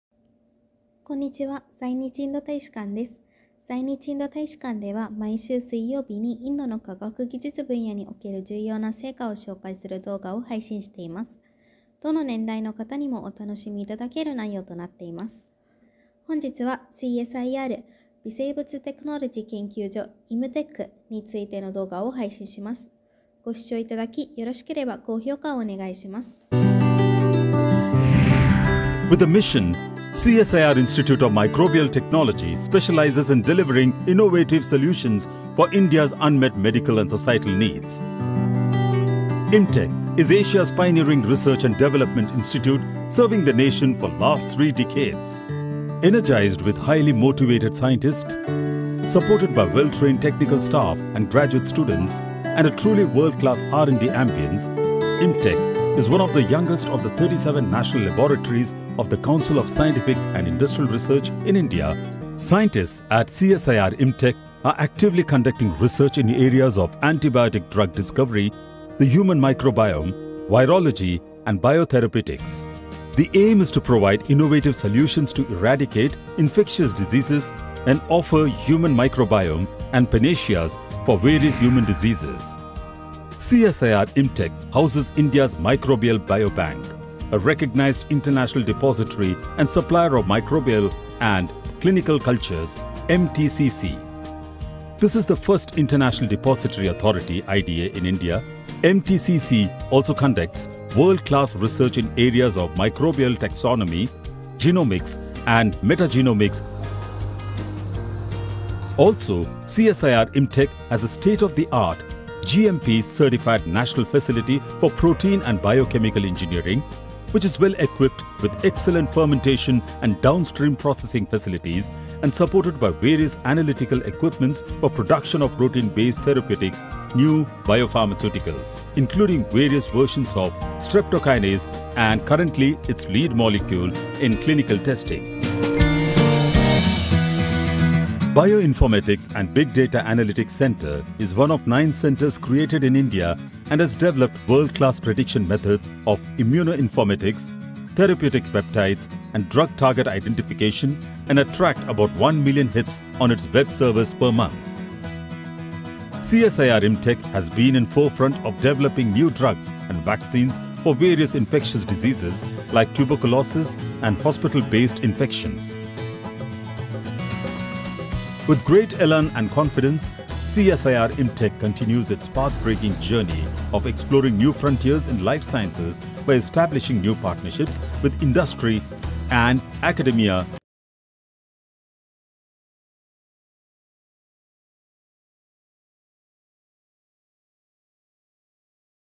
CSIR IMTECH
India Science & Technology series: Watch a short video on CSIR-Institute of Microbial Technology (IMTech), one of the 38 national laboratories of the Council of Scientific & Industrial Research CSIR, India; an International Depository Authority for Microorganism.